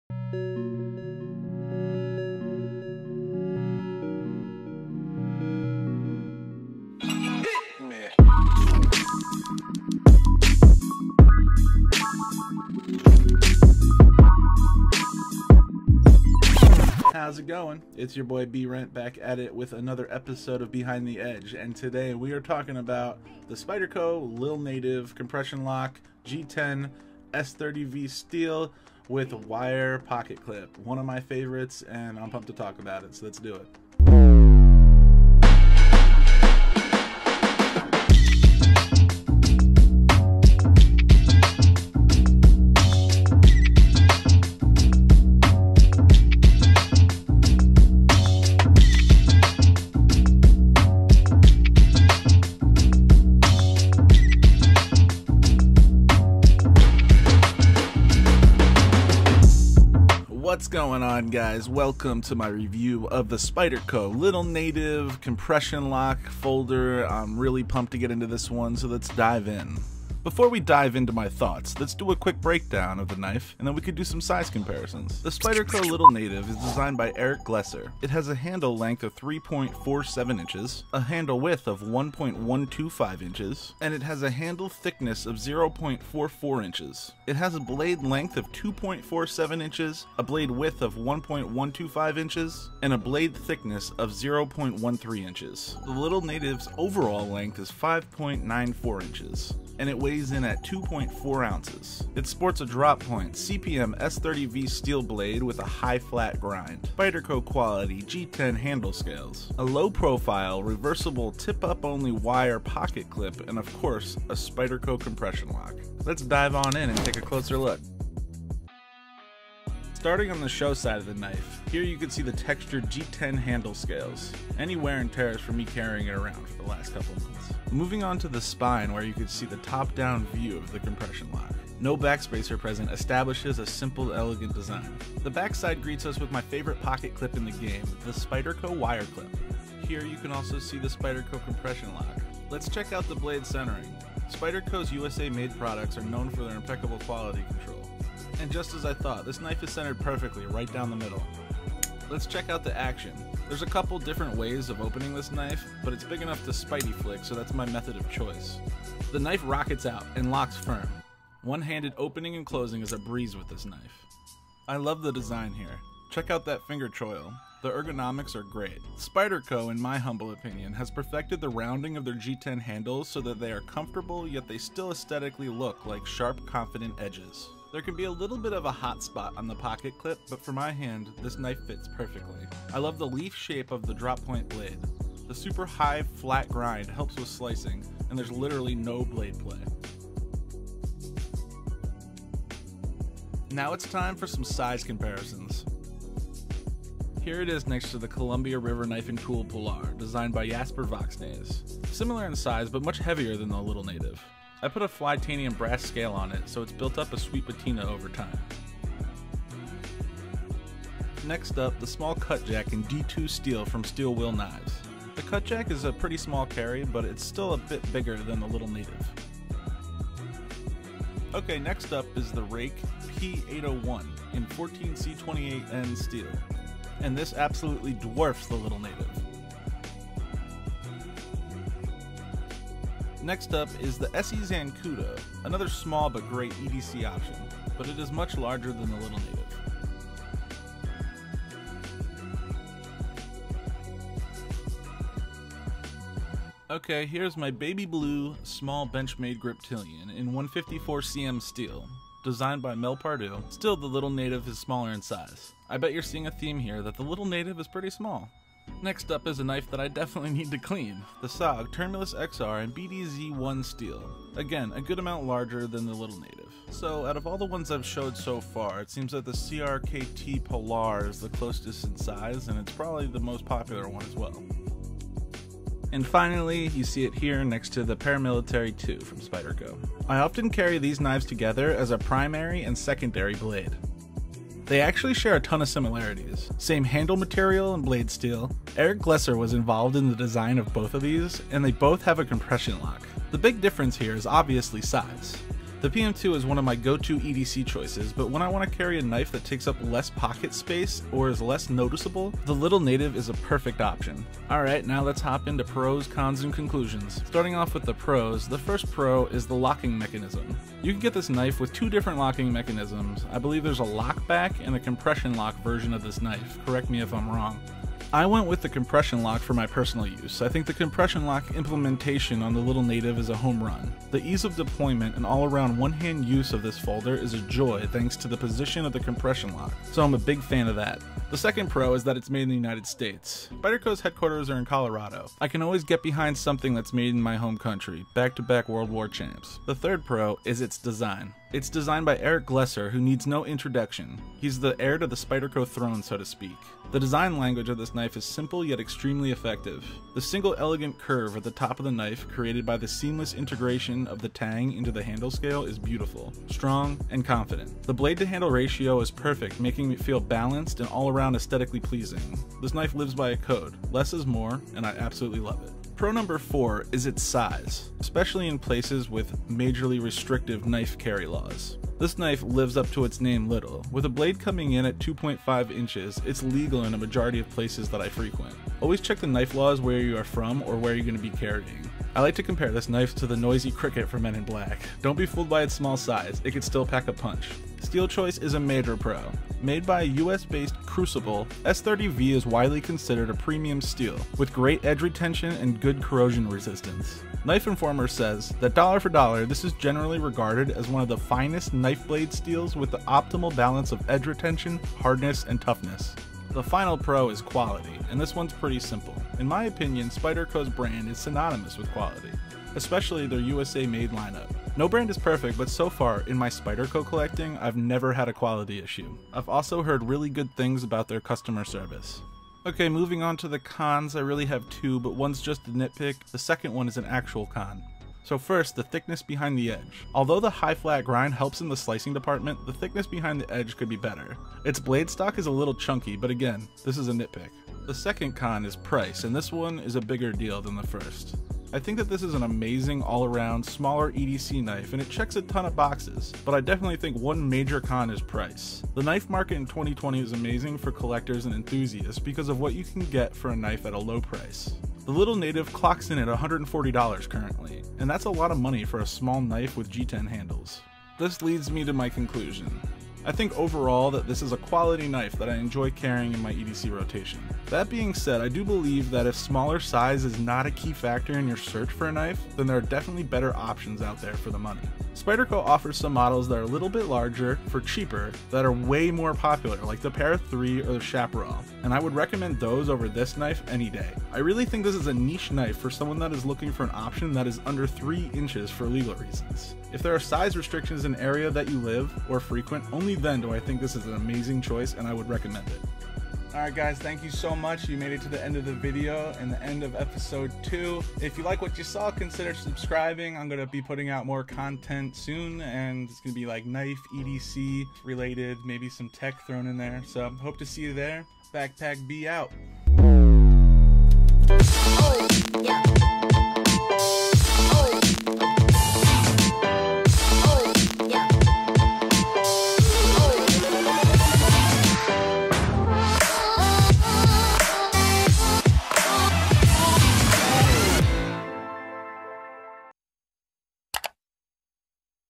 The Spyderco Lil' Native: Behind The Edge Episode 2
A niche knife that I have grown to love over the past couple months.  This video covers my thoughts on the Spyderco Lil' Native.

What are your thoughts?  Did I get anything wrong?

Behind the Edge will eventually be a weekly show I put out every Wednesday!